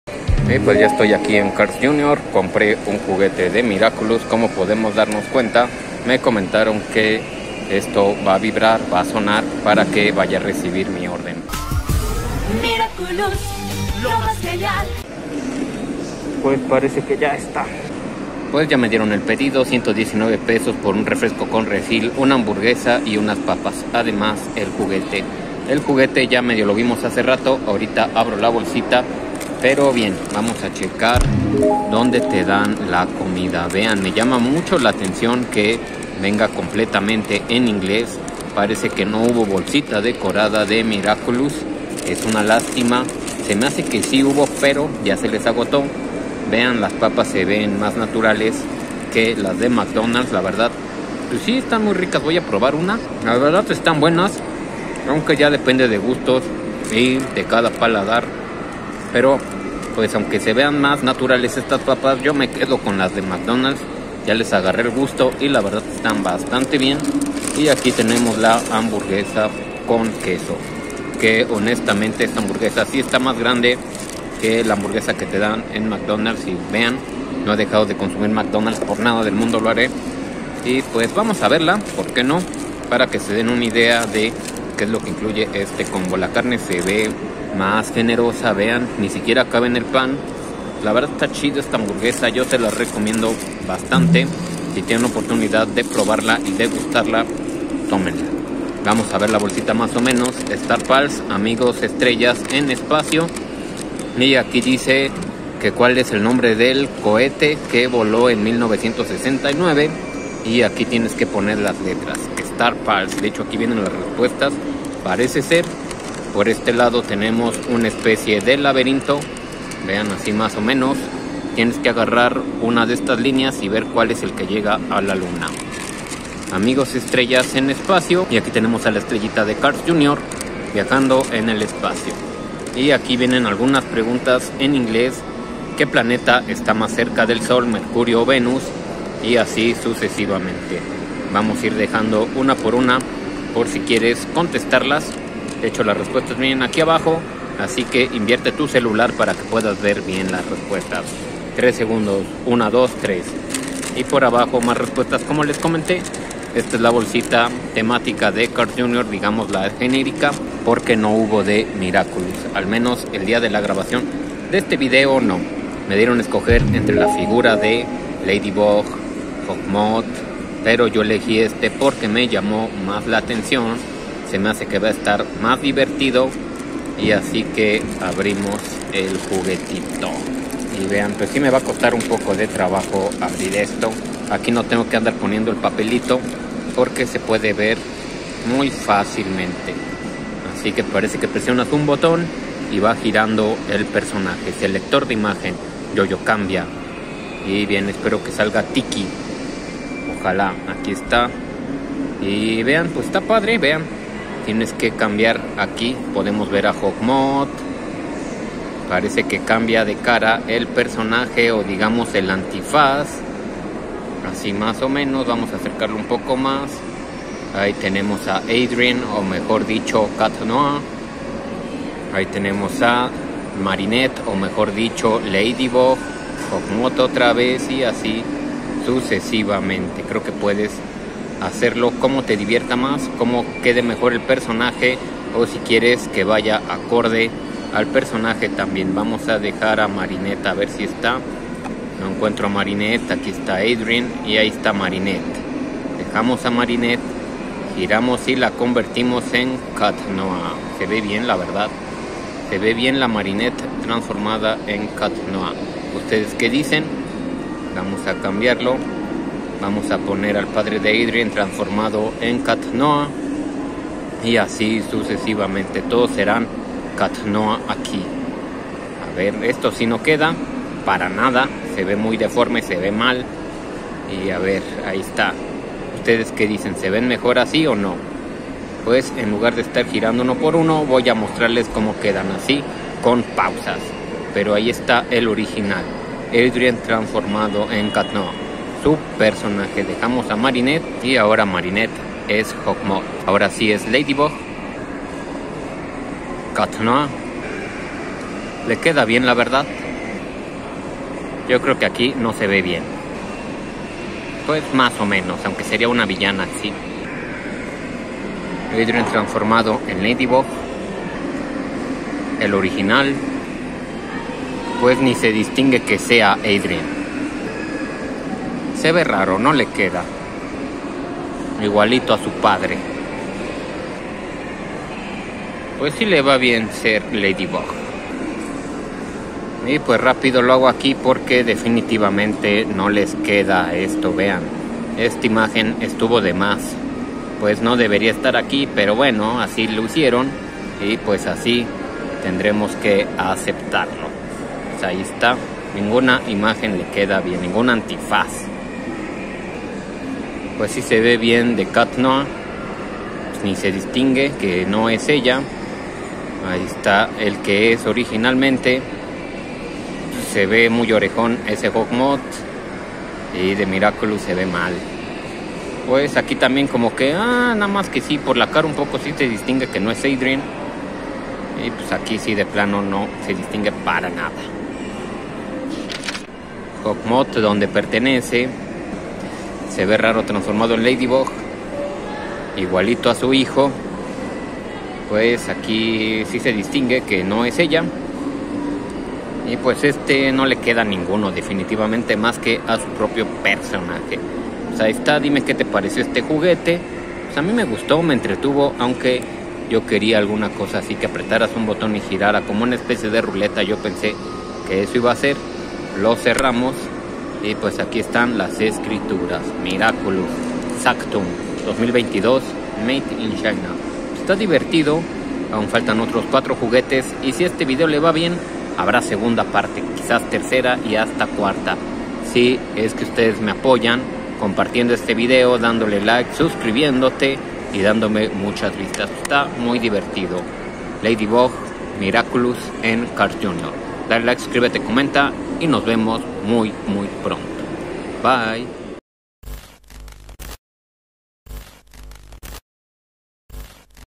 Miraculous: Las aventuras de Ladybug Juguetes En Carls Jr Star Pals (Junio/Julio 2023)
Son 5 juguetes disponibles en el combo star pals de Miraculous: Las aventuras de Ladybug los cuales son: LadyBug (Marinette) , Cat Noir (Adrien) , Hawk Moth , Yoyo Cambia Personaje, Tikki y Plaga